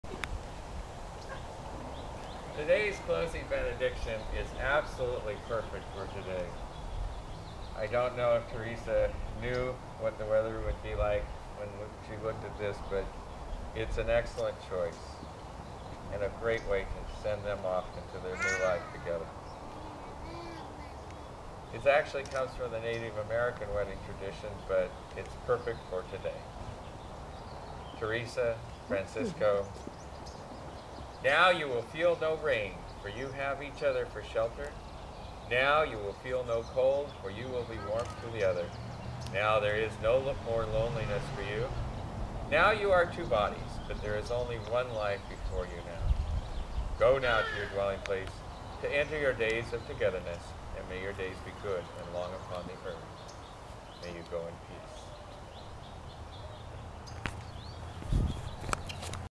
Mar 03 2009 - VID00014.AVI
Wedding vows, conducted by wedding officiant 'Roc N Rev' Michael Taylor at a Central Coast of California wedding venue.